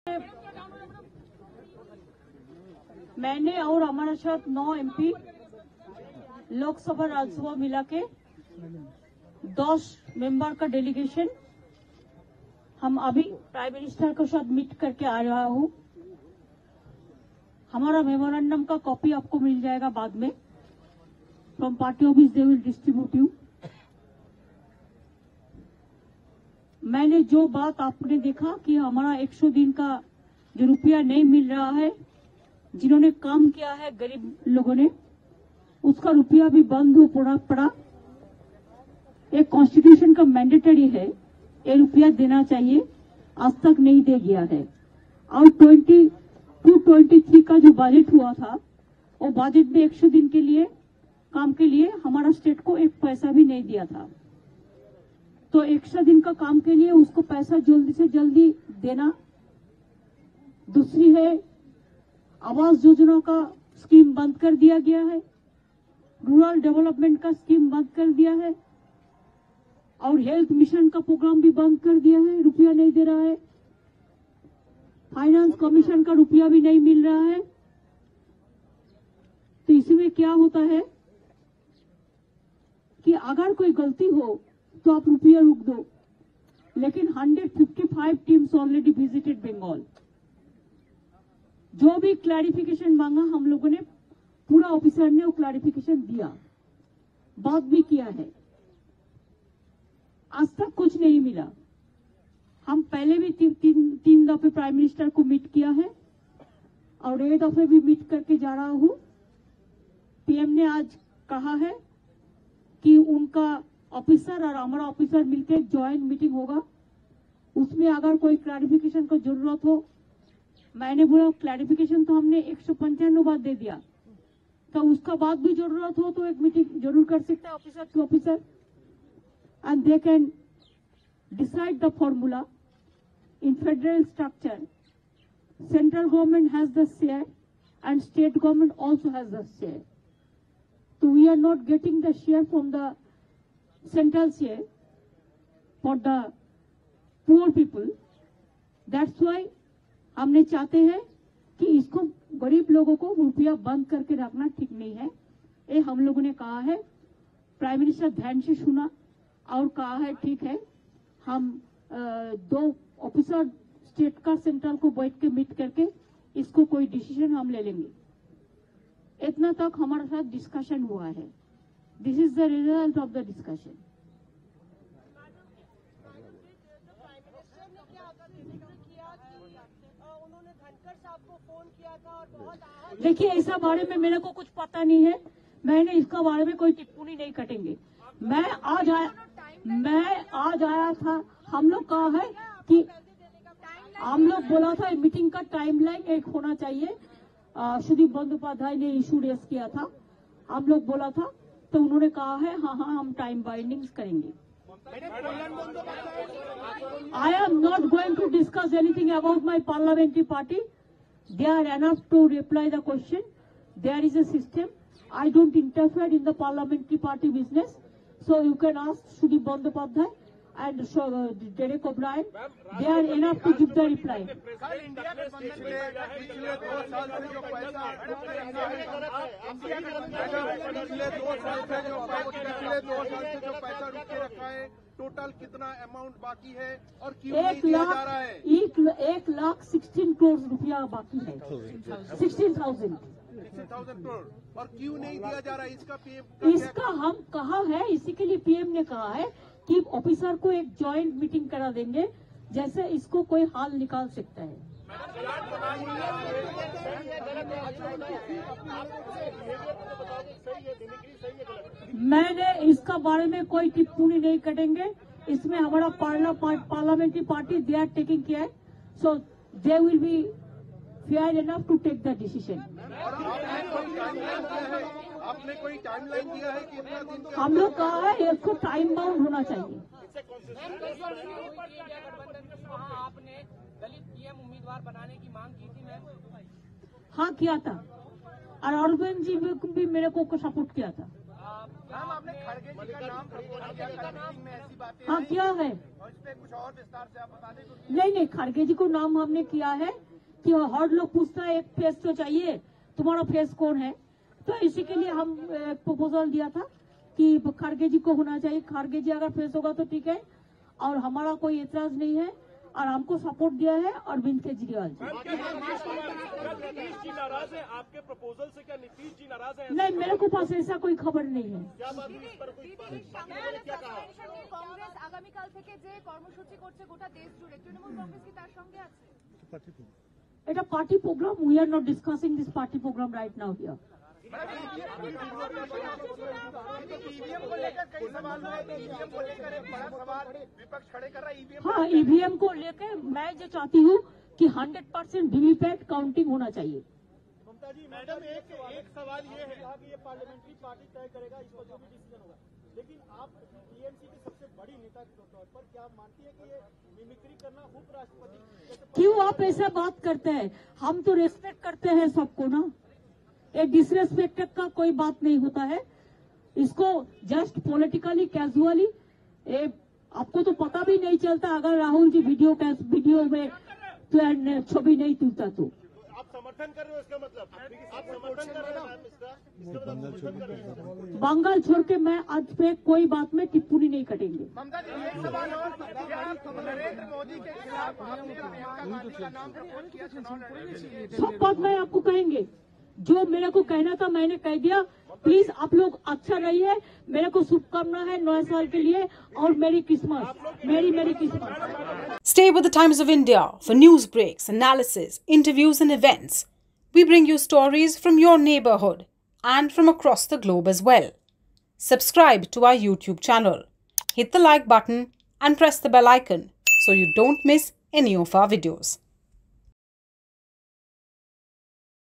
Mamata Banerjee upset with PM Modi | "Centre has held funds for West Bengal, for our poor people"
After meeting with Prime Minister, Narendra Modi, West Bengal Minister Mamata Banerjee, along with a delegation of 11 MPs address media.